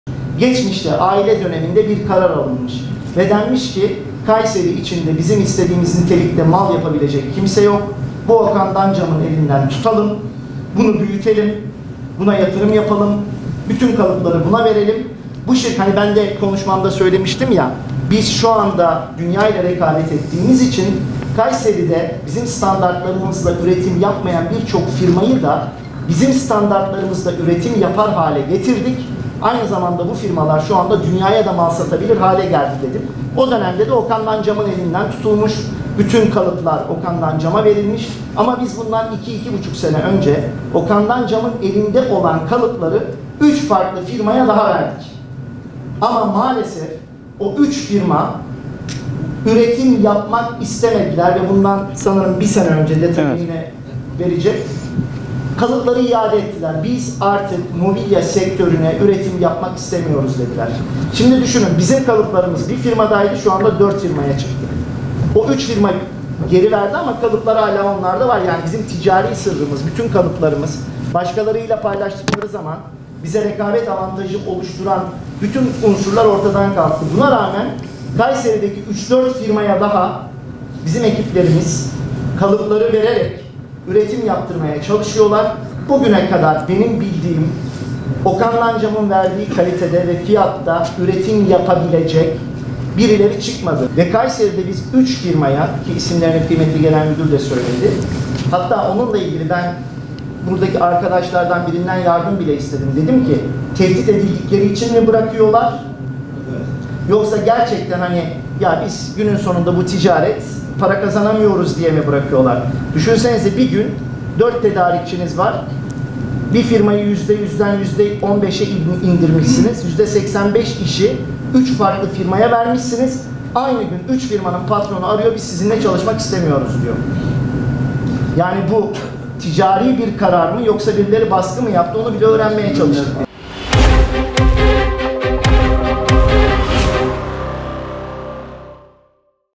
Erciyes Anadolu Holding CEO'su Alpaslan Baki Ertekin ' Aynı Gün 3 Firma Bizimle Çalışmak İstemedi.'
Okandan Cam ile ilgili olarak da şuanda Okandan Cam'ın hissedarı ortakları arasında FETÖ ilgili ceza almış yargılanan kimsenin olmadığını ifade eden Erciyes Anadolu Holding CEO'su Alpaslan Baki Ertekin ,' Geçmişte bir karar alınmış ve denmişki Kayseri'de bizim istediğimiz nitelikte mal yapacak kimse yok bu Okandan Cam'ın elinden tutalım, bunu büyütelim buna yatırım yapalım, bütün kalıpları buna verelim. Bende konuşmamda söylemiştim ya biz şuanda dünya genelinde rekabet ettiğimiz için Kayseri'de bizim standarlarımız da üretim yapmayan birçok firmayı da bizim standartlarımız da üretim yapar hale getirdik. Aynı zamanda bu firmalar dünyaya mal satabilir bir hale geldi. O dönemde de Okandan Cam'ın elinden tutulmuş bütün kalıplar Okandan Cam'a verilmiş. Ama biz bundan 2-2,5 sene önce Okandan Cam'ın elinde olan kalıpları 3 farklı firmaya daha verdik. Maalesef o 3 firma üretim yapmak istemediler ve bundan 1 sene önce kalıpları iade ettiler. Biz artık mobilya sektörüne mobilya üretimi yapmak istemiyoruz dediler. Şimdi düşünün biim kalıplarımız 1 firmadaydı şuanda 4 firmaya çıktı. O 3 firma geri verdi ama kalıplar onlarda. Bunlar bizim ticari sırrımız. Başlarıyla paylaştıkları zaman bize rekabet avantajı oluşturan bütün oluşumlar ortadan kalktı. Buna rağmen Kayseri'de ki 3-4 firmaya daha bizim ekiplerimiz kalıpları vererek üretim yaptırmaya çalışıyorlar. Bugüne kadar benim bildiğim Okandan Cam'ın verdiği kalitede ve fiyatta artık üretim yapabilecek birileri çıkmadı.' diyerek aynı gün 3 farklı firmanın kendileriyle çalışmak istemediklerini söylediğini ifade etti.